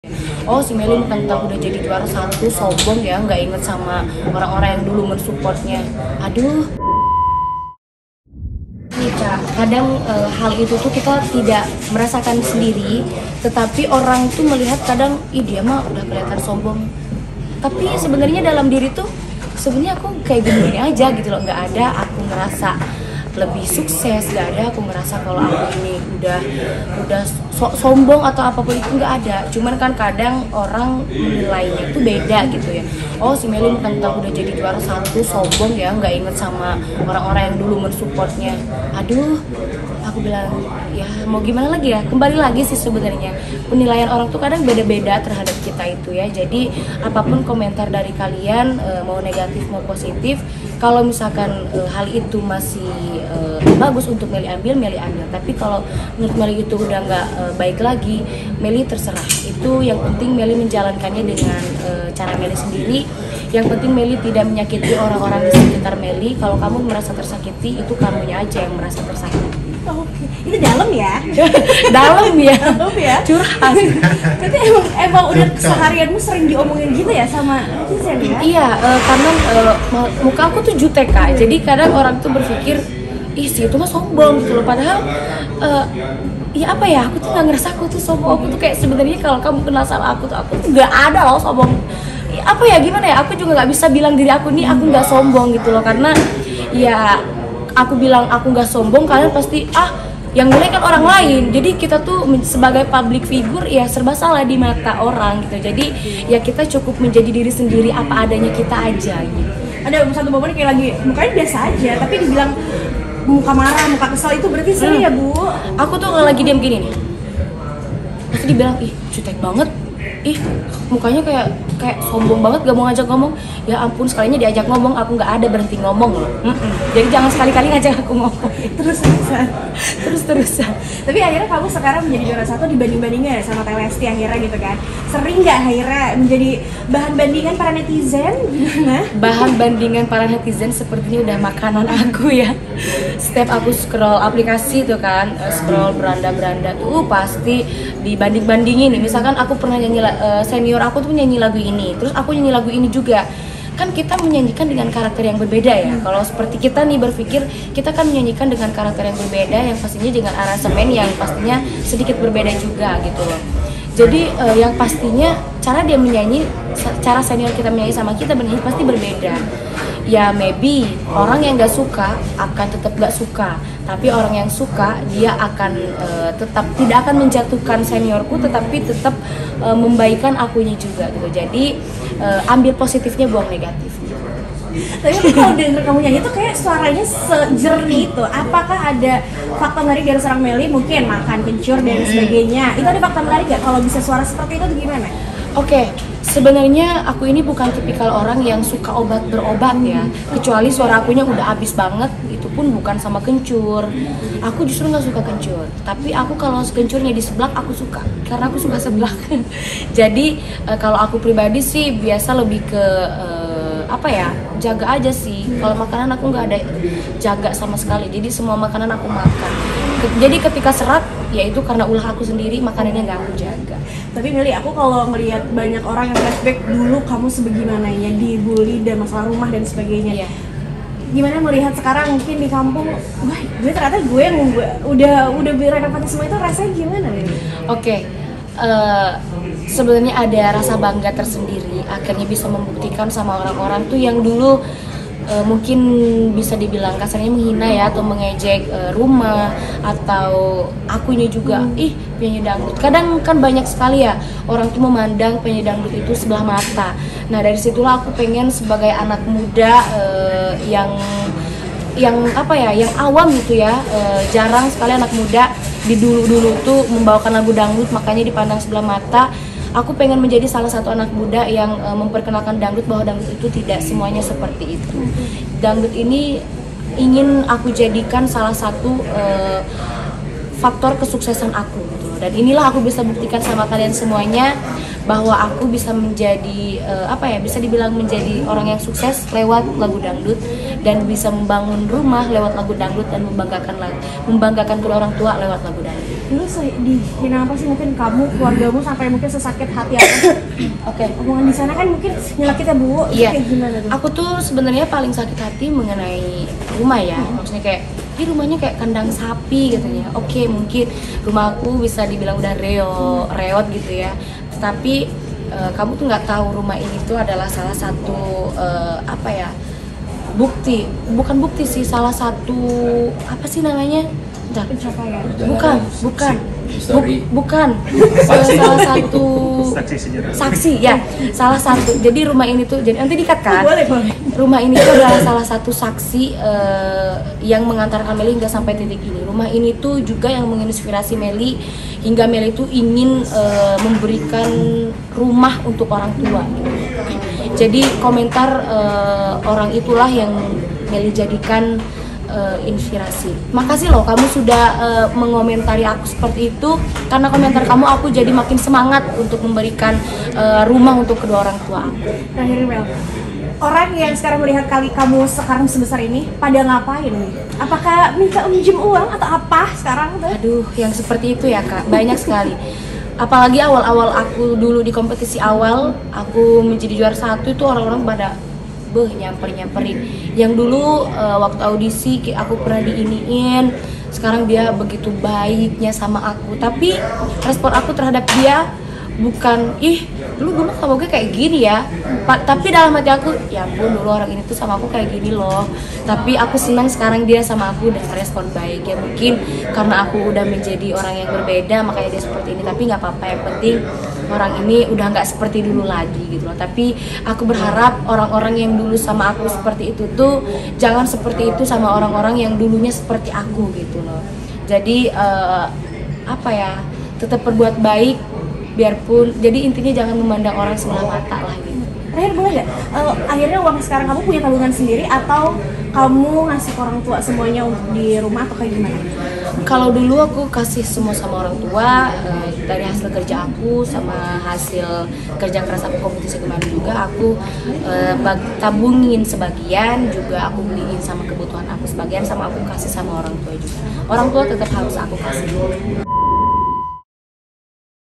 Melly Lee Juara D’Academy Asia Santai Di Cap Sombong
mellylee raih juara 1 d’avademy asia